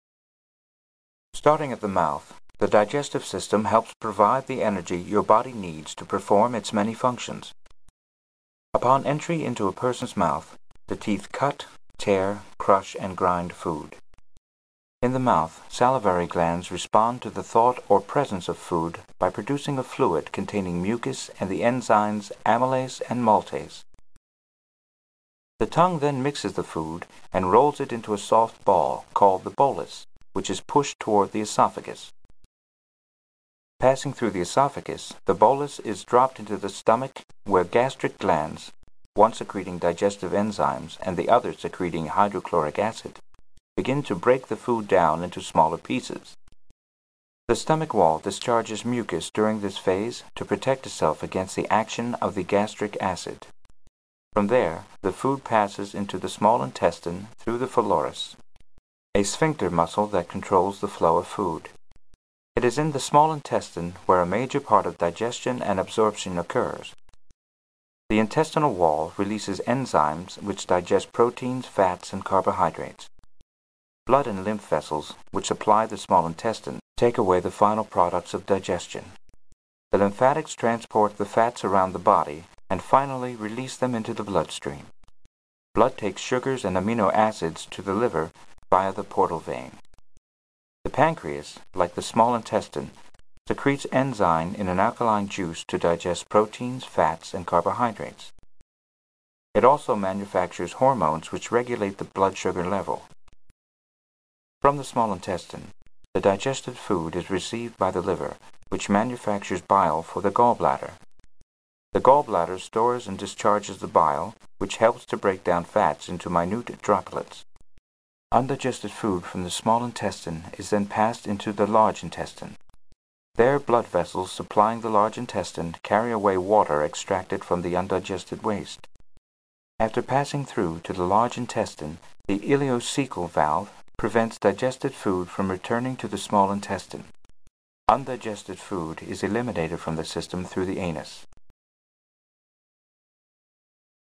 How the Body Works The Digestive System
When you eat foods—such as bread, meat, and vegetables—they are not in a form that the body can use as nourishment. Food and drink must be changed into smaller molecules of nutrients before they can be absorbed into the blood and carried to cells throughout the body. Digestion is the process by which food and drink are broken down into their smallest parts so the body can use them to build and nourish cells and to provide energy.